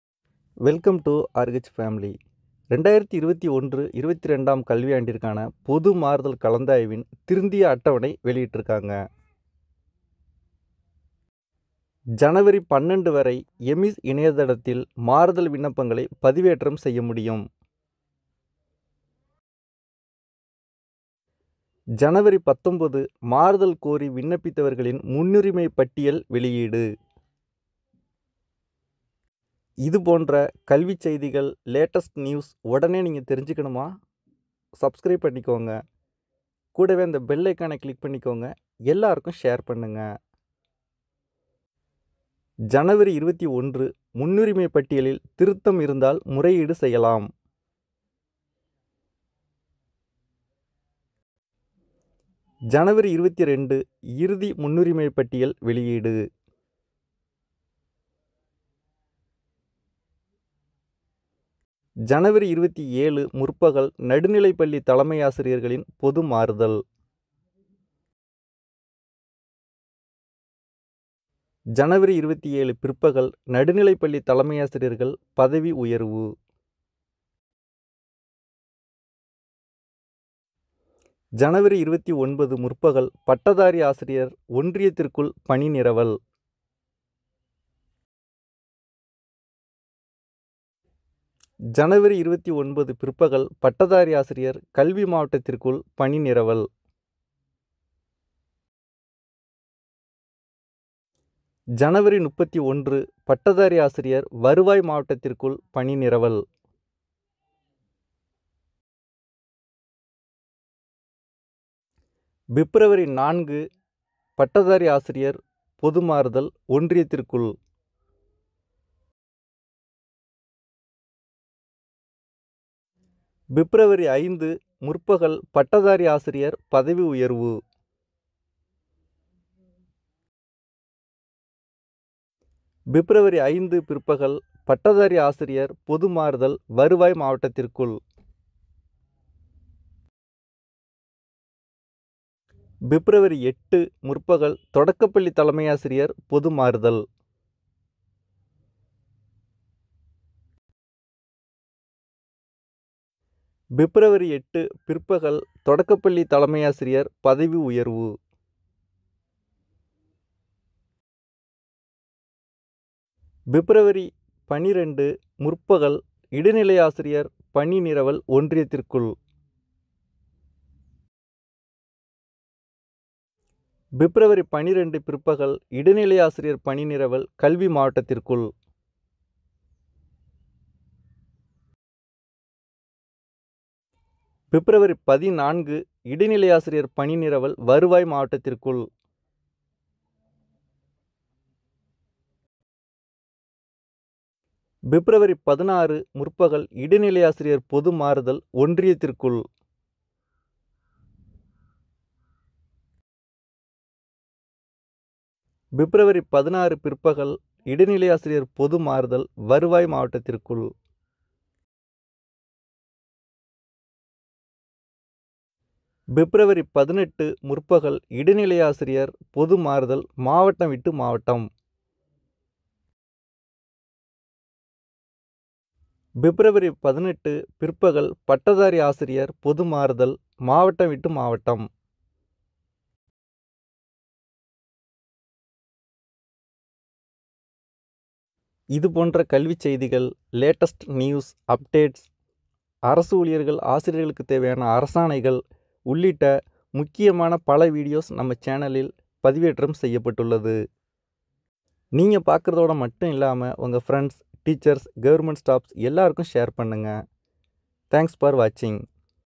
361. புதிய திருத்திய அட்டவணை * ஆசிரியர் பொதுமாறுதல் கலந்தாய்வு
புதிய  திருத்திய  அட்டவணை  வெளியீடு..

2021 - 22.. ஆசிரியர்  பொதுமாறுதல்  கலந்தாய்வு

முழு  பட்டியல்..

NEW  REVISED  TIMETABLE
RELEASED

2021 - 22
TEACHERS
TRANSFER  COUNSELING